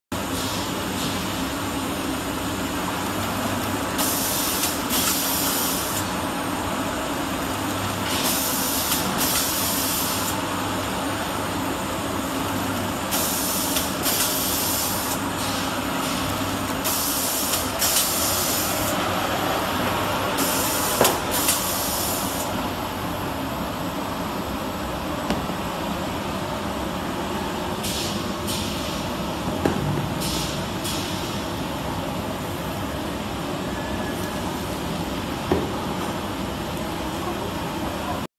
Automatic box shrink wrap packing machine
Automatic L-shaped sealing and cutting shrinking machine:

Model

FQL-450

FQL-550

Packing Material

PO,POF or PE three layer extruded shrink film roll

Power Supply

380V/50HZ or 220V/60HZ 3PH

380V/50HZ or 220V/60HZ 3PH

Total Power

8.5KW

12.35KW

Sealing Length＆Width

560*320*125mm(L*W*H)

650*420*125mm

Contract the speed

0-10m/min

0-10m/min

Produce the ability

20-30 packs/min

20-30 packs/min

Thickness*width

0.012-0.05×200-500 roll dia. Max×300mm

Air Consumption

0.5MPA

0.5MPA

Overall Dimensions

4000*850*1400mm(L×W×H)

4500*850*1400mm

Weight

550kg